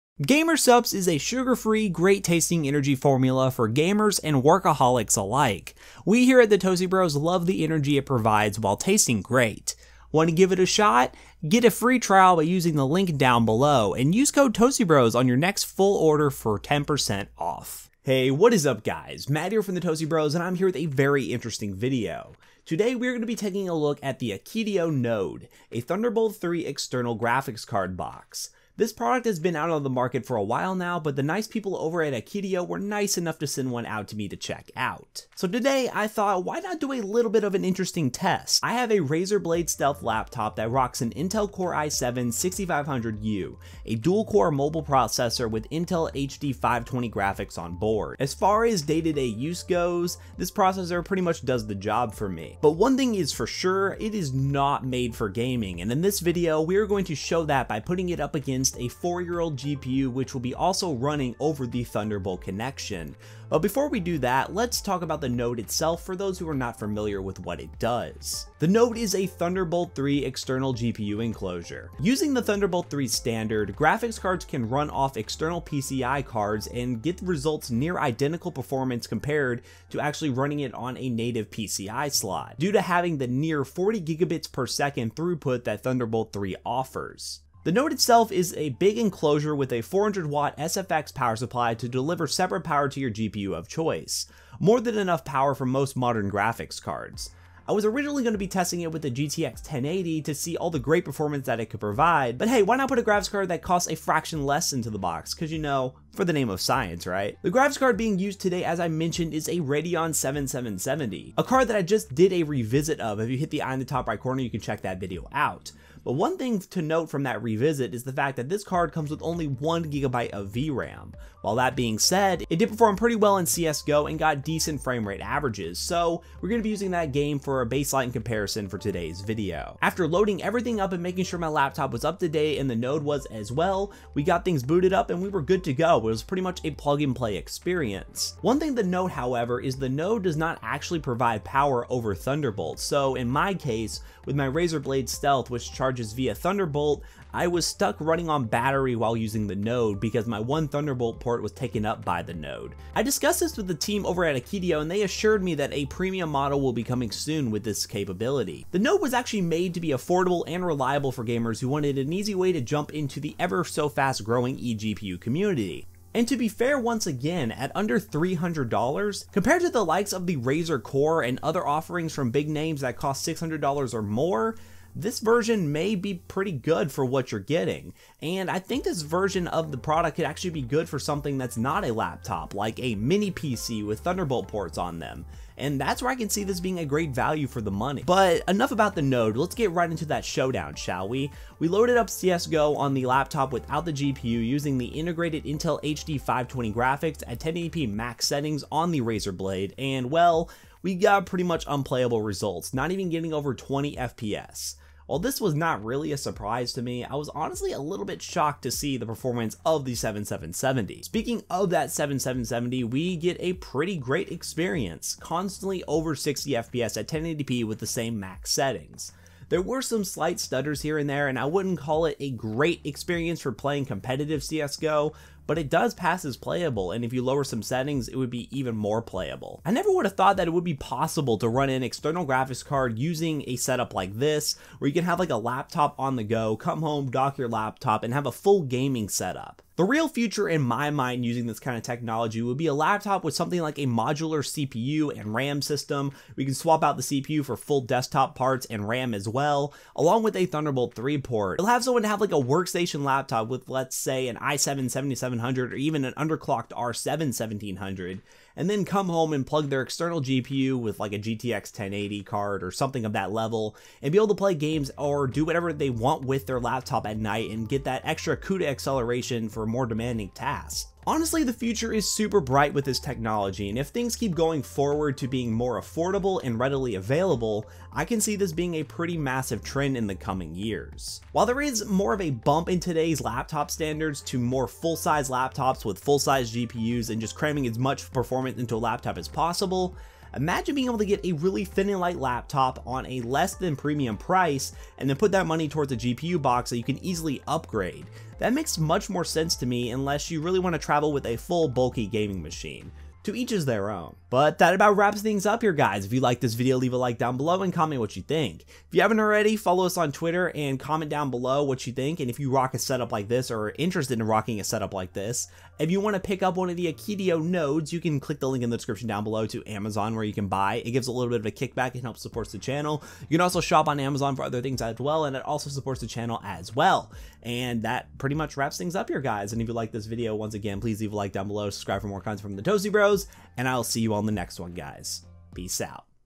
4 Year Old GPU vs Intel Integrated Graphics - Akitio Node Review
The Akitio Node is an external gpu box with thundebolt 3 support. But how does a Radeon 7770 compare vs the i7 6500u and its Intel hd 520 graphics? Let's test it!

Akitio Node

We as the toasty bros strive to create content directed to please our viewers. Our passion for tech becomes your passion with every video.